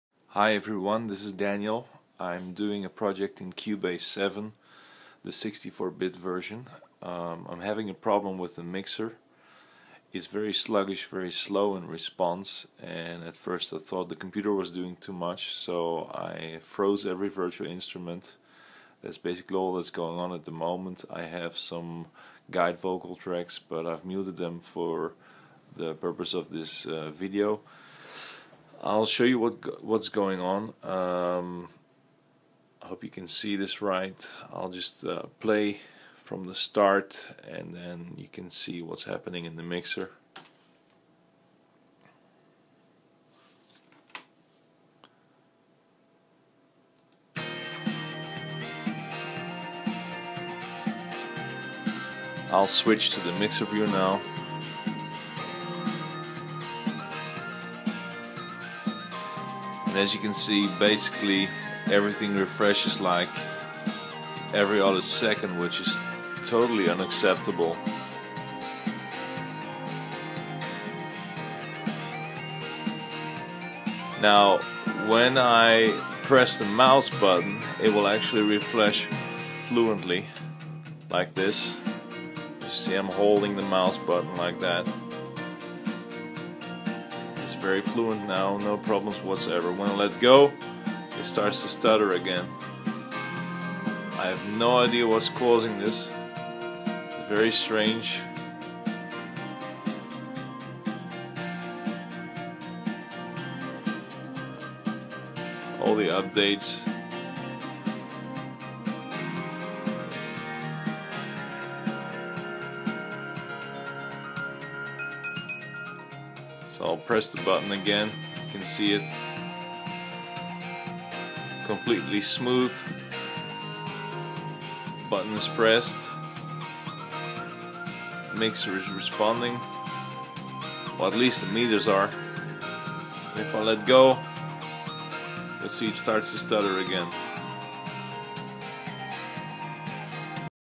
Cubase 7 64 bit Mixer Sluggish/Slow Refresh and handling
This is a short video to demonstrate the problem I'm having with the mixer in Cubase 7 64bit version. When I load the same song in Cubase 7 32 bit version there is no refresh issue whatsoever, everything updates and handles smooth. This is running on a Windows 7 64 OS. Is this a bug?